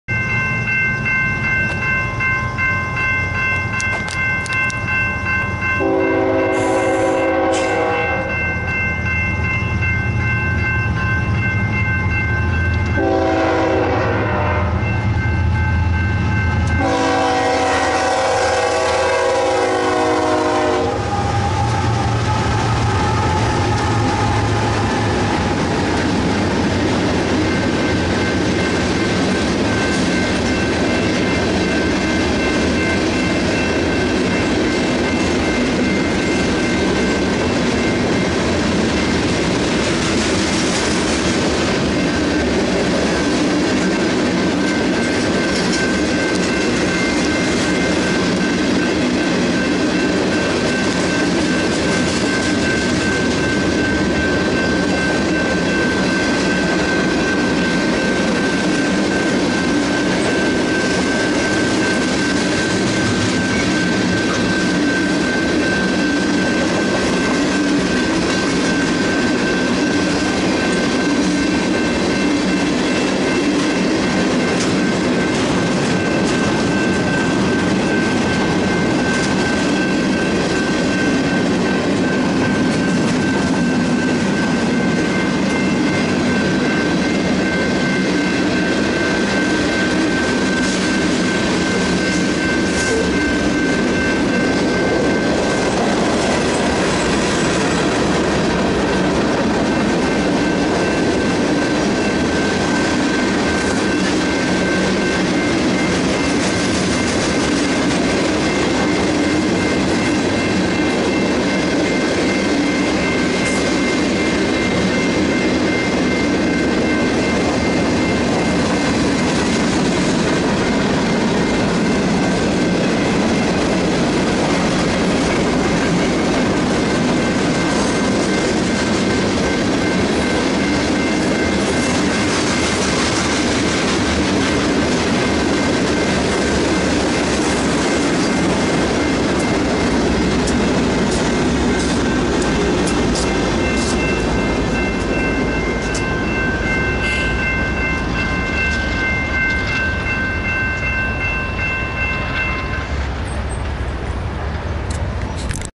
BNSF Mixed Freight Train with 2 DPU
BNSF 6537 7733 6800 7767 leads a west bound mixed freight through Santa Fe Springs with 2 DPU BNSF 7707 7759.